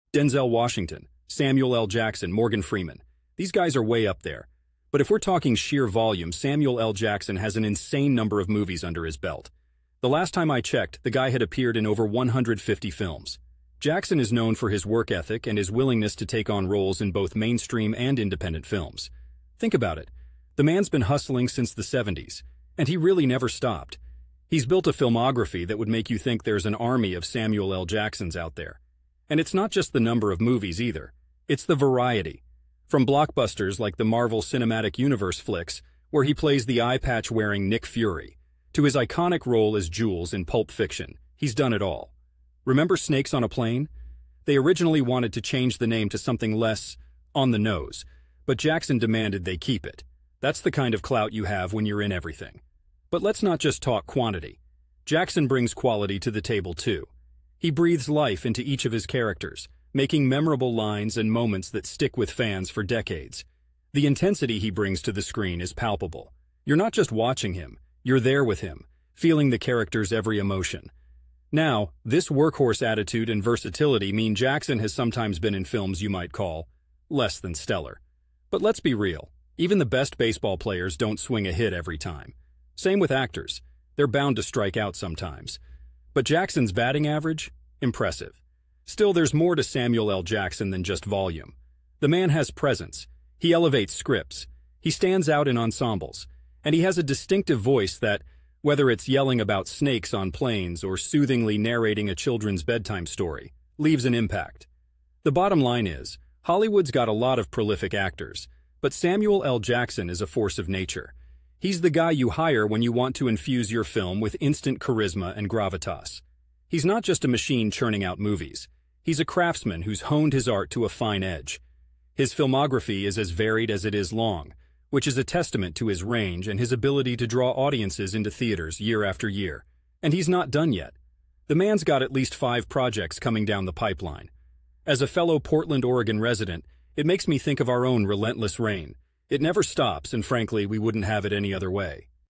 What Black Actor Has The Most Movies?
I answered this question on Quora My video answer to the question: What Black Actor Has The Most Movies?